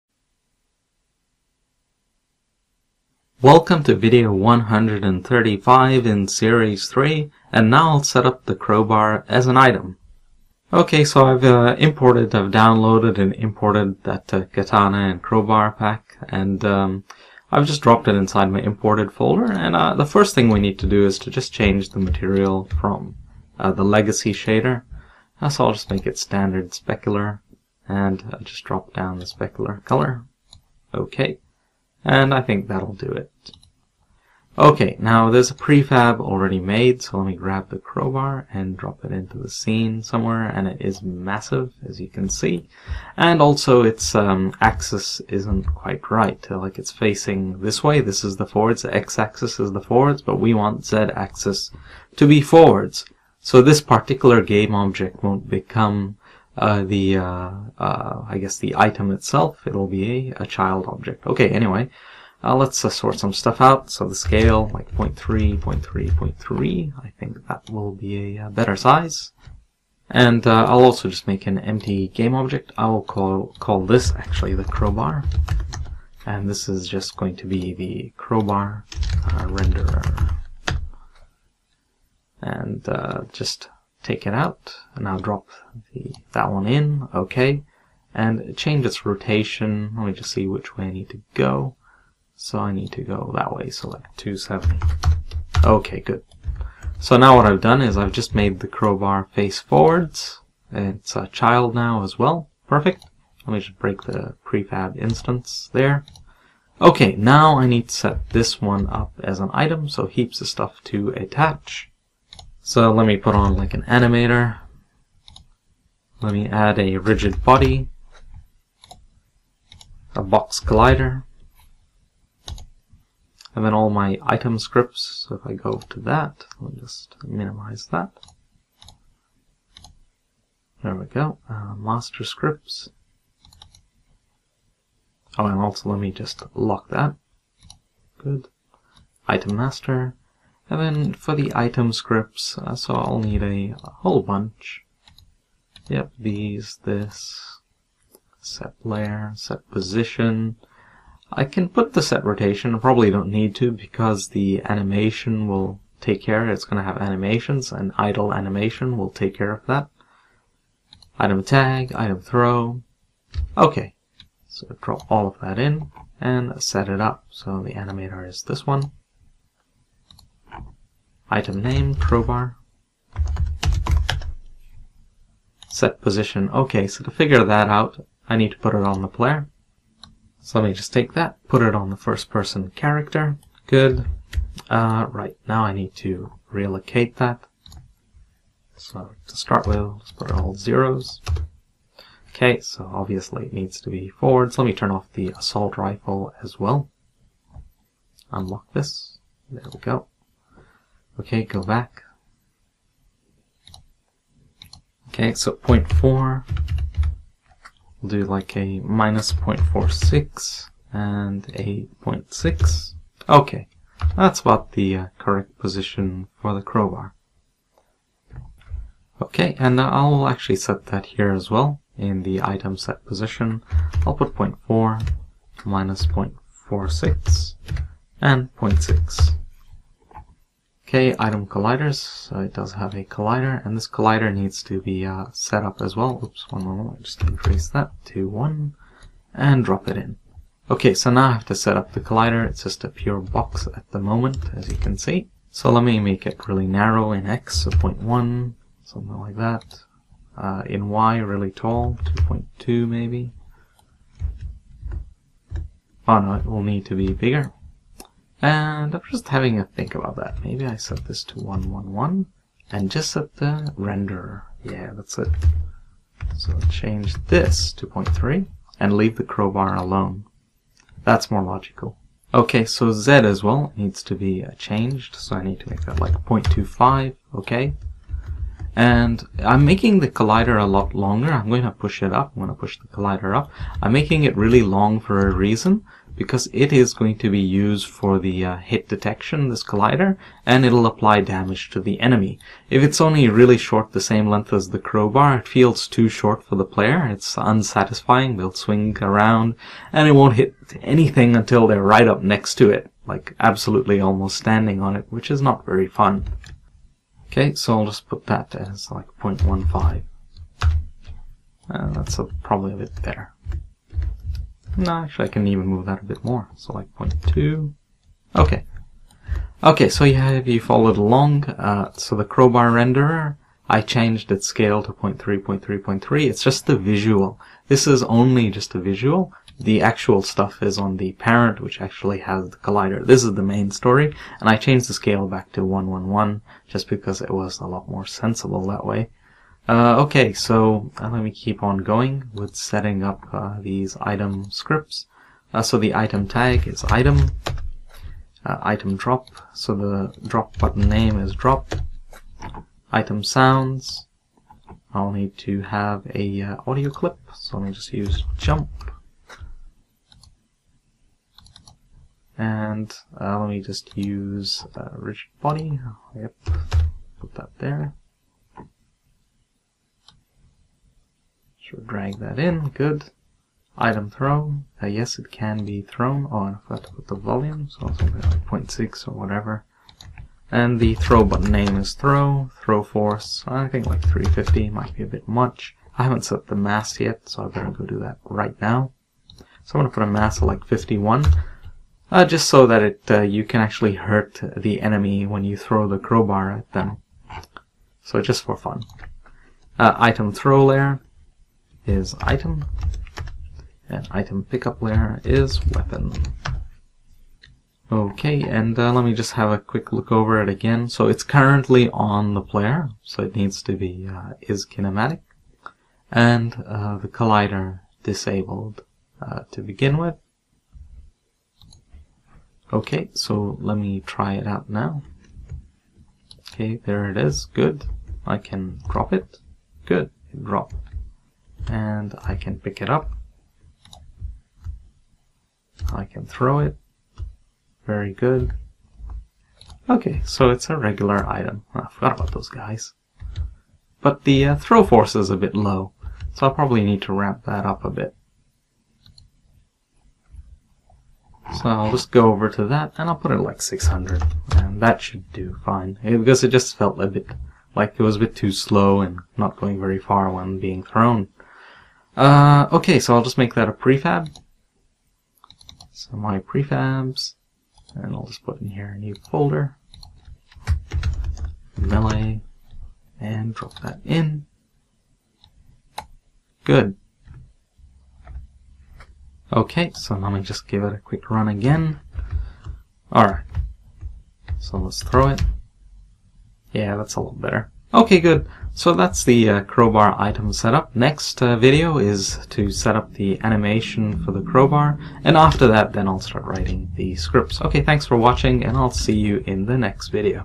[135] Setup Crowbar - How To Make A Game
I'll use a crowbar as an example melee weapon and the first step is to set it up as an item.